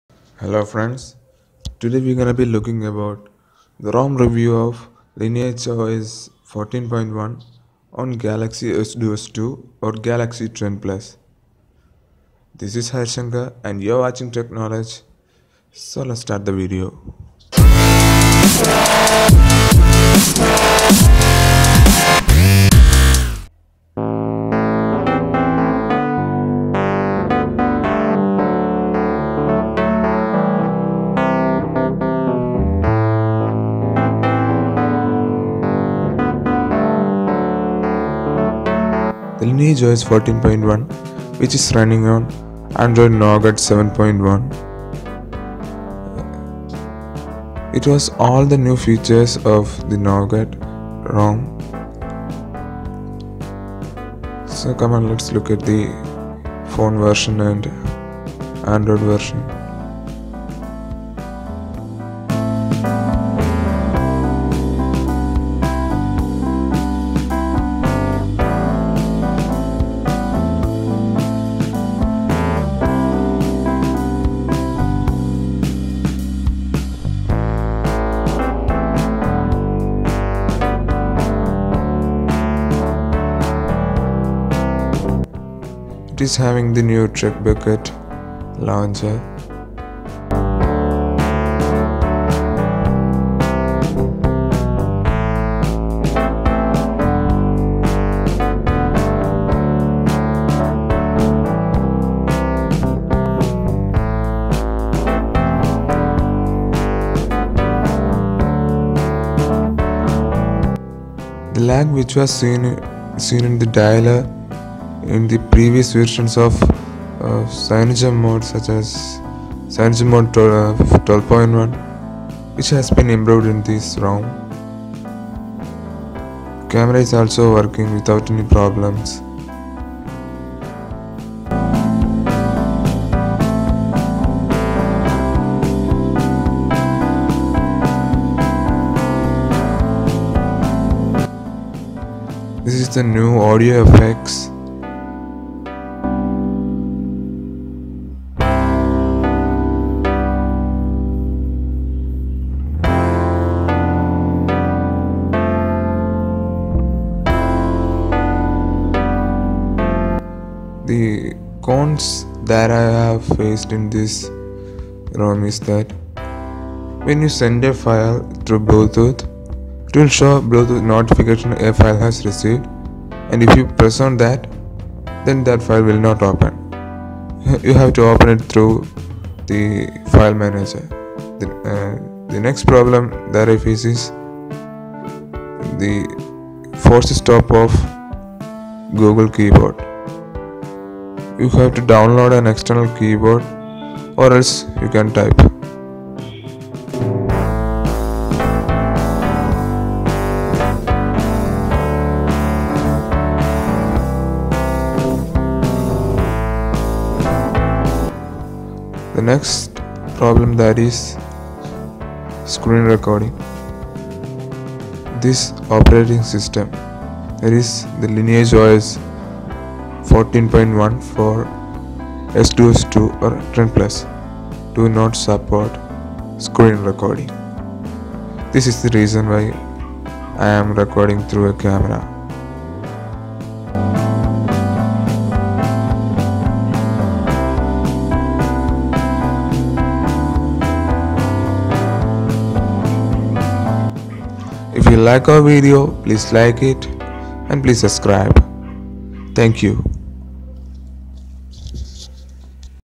Lineage OS 14.1 (Nougat 7.1.1) On Galaxy S Duos 2/Galaxy Trend Plus (GT 7580 or GT 7582)
In this video we will you the rivew of lineage OS 14.1 based on android nougat which runs on either Samsung galaxy trend plus or galaxy s duos 2. We will show and tell you about the negatives and positives of the ROM.
Link to download Lineage OS 14.1-